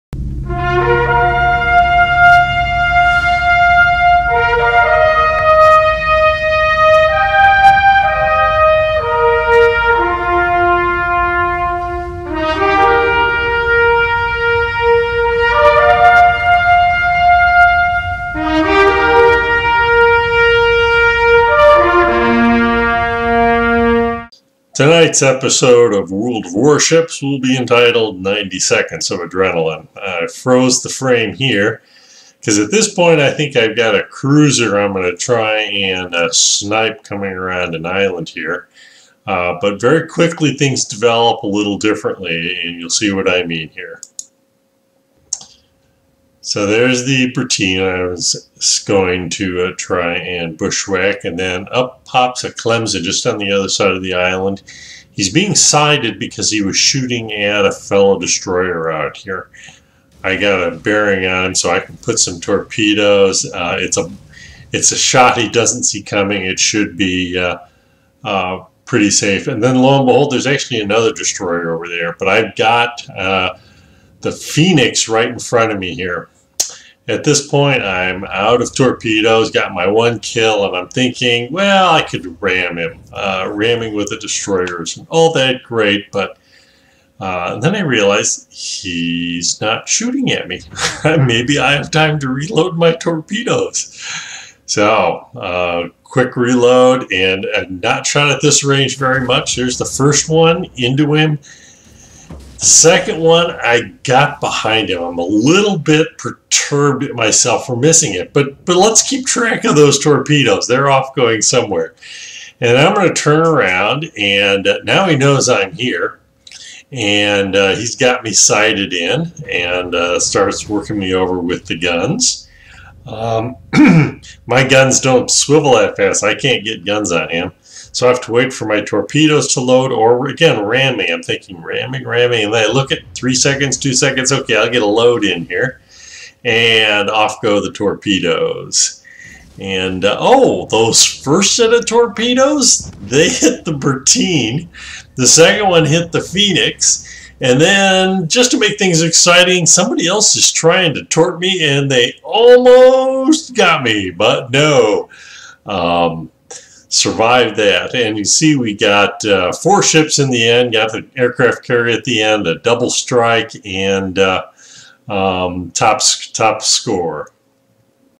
90sec
90 seconds of C9 H13 NO3. Three sunk in 90 seconds including two devastating strike and a double strike. The words of Lefty Gomez would seem to apply.